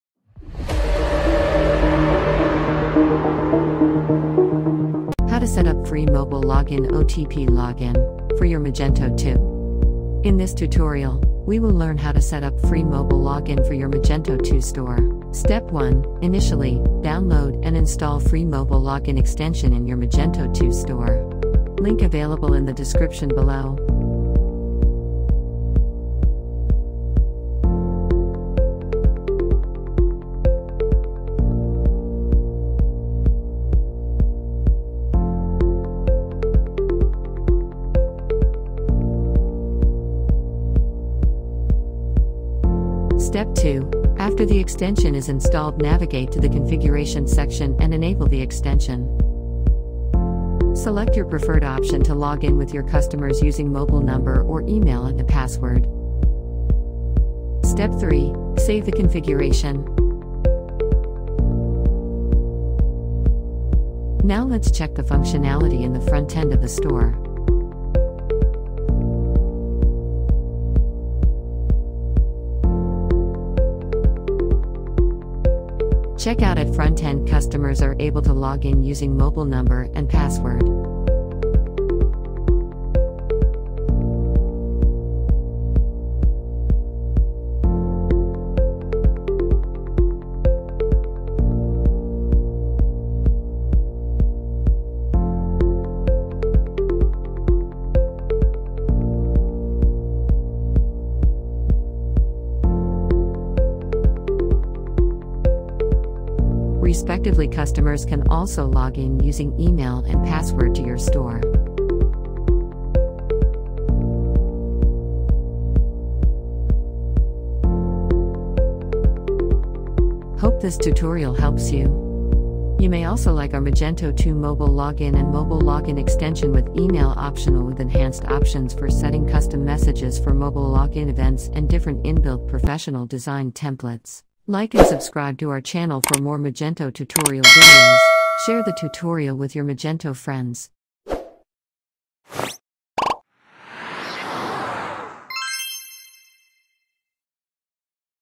How to Setup FREE Mobile Login For Your Magento 2 (OTP Login)
Lets store customers quickly login through their mobile number with the layer of security by sending a One Time Password (OTP) for login.

Adding such mobile login functionality to your Magento 2 store helps the customers to save their time by quickly logging in or register using their mobile number.